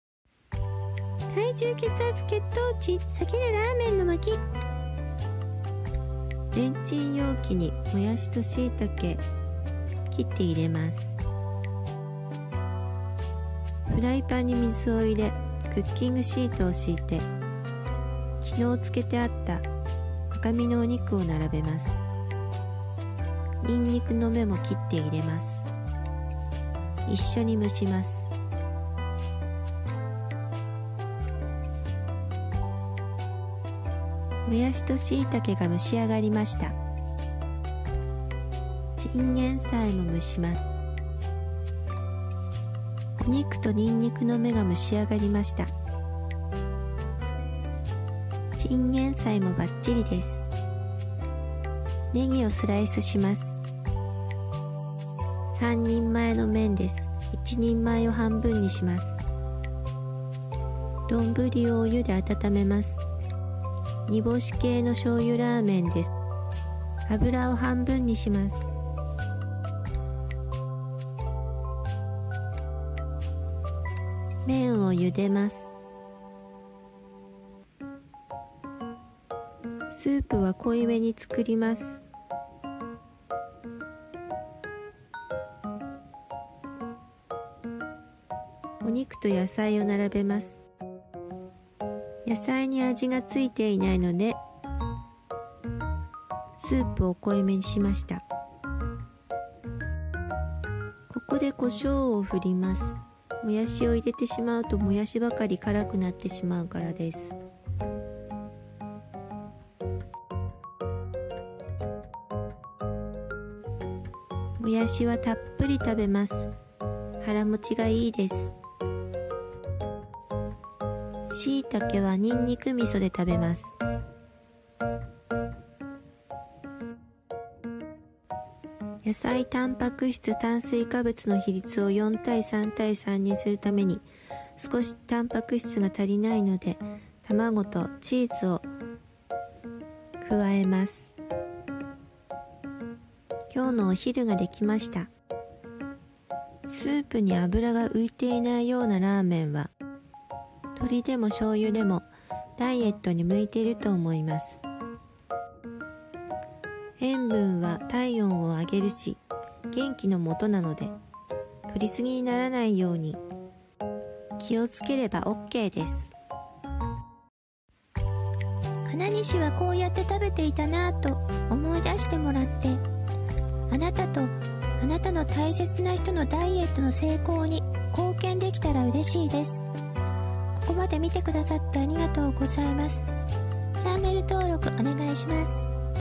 野菜たっぷりラーメン 体重・血圧・血糖値下げる
痩せた人はどうやって何を食べたのか？昔一番知りたかったことです。わたしはこういうものをこうやって食べて痩せました。参考になれば幸いです。
注意：お酒に弱い人、お子様の食事には料理酒か水を使ってください、酔っぱらいます
「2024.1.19」食べたもの記録　瘦せるラーメン　japanese healthy diet I want to lose weight